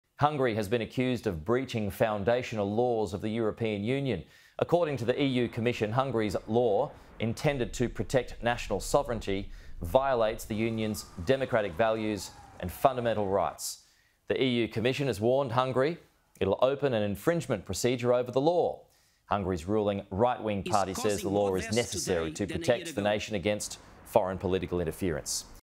EU to initiate legal action against Hungary over ‘sovereignty law’
Hungary has been accused of breaching foundational laws of the European Union including democracy.

The EU commission particularly took aim at Hungary's law which intends to 'protect national sovereignty'.

The EU says the law violates the union's democratic values and fundamental rights.

The EU commission has warned Hungary it will open an infringement procedure over the law.

Hungary's ruling right wing party says the law is necessary to protect the nation against foreign political interference.